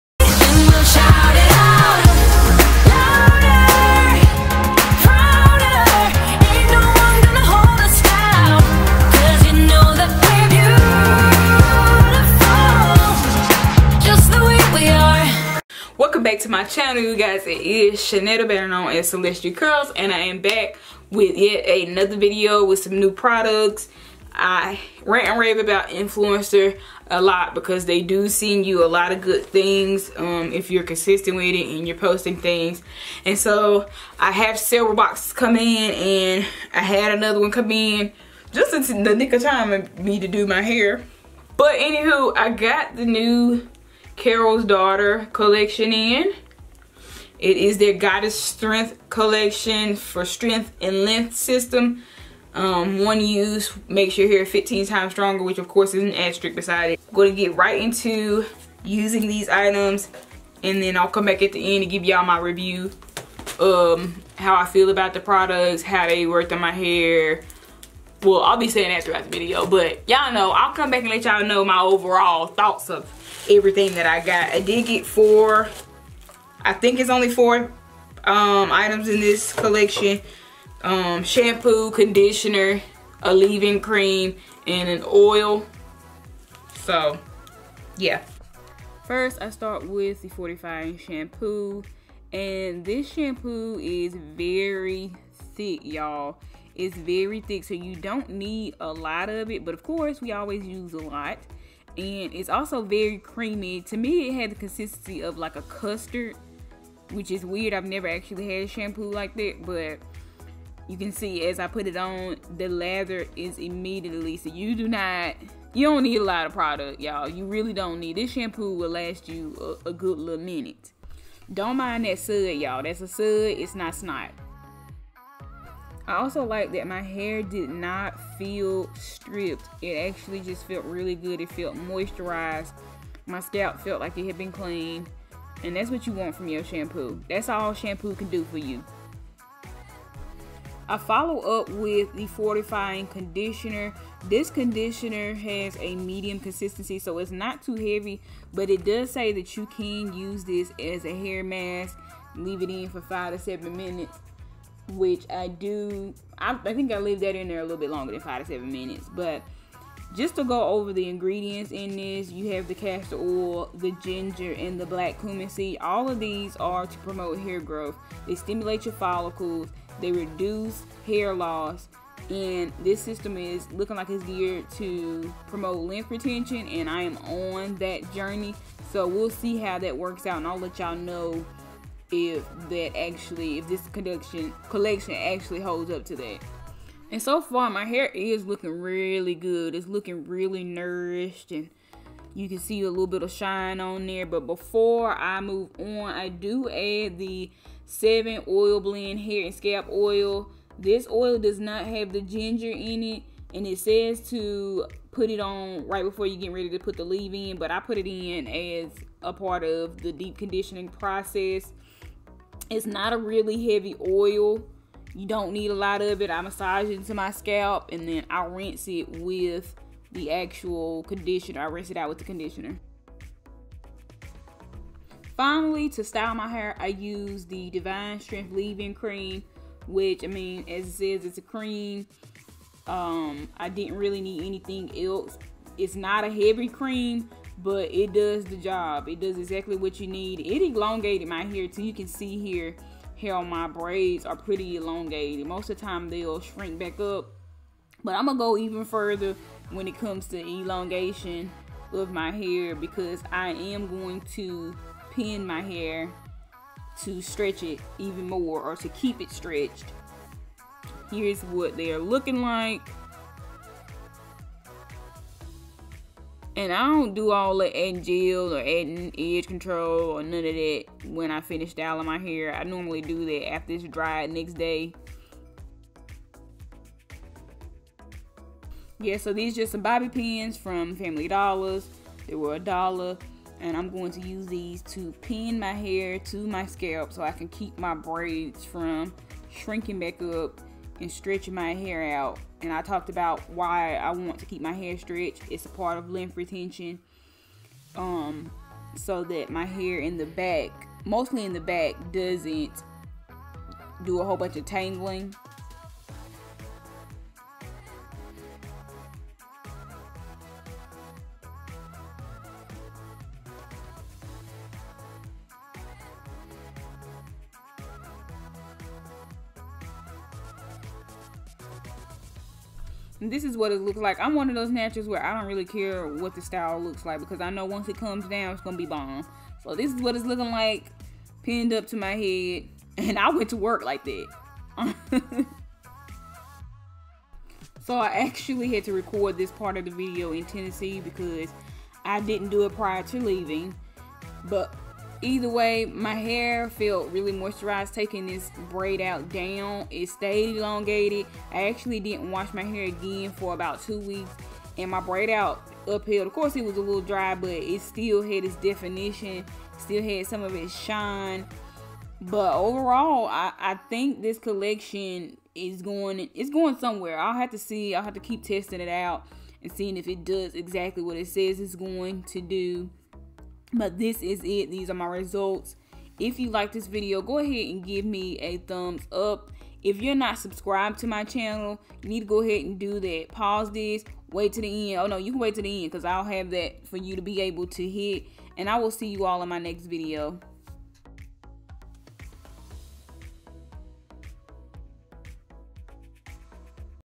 Carols Daughter Goddess Strength
I have been doing a lot of stretched styles on my hair lately to help with my length retention and still be able to give you all content without compromising my hair. In this video I am using Carol's Daughter Goddess Strength Collection sent to me complimentary by Influenster to give my honest review which I always do. This is another collection that again requires use over time to see if it does what it claims!

GODDESS STRENGTH SHAMPOO, CONDITIONER, LEAVE-IN AND HAIR OIL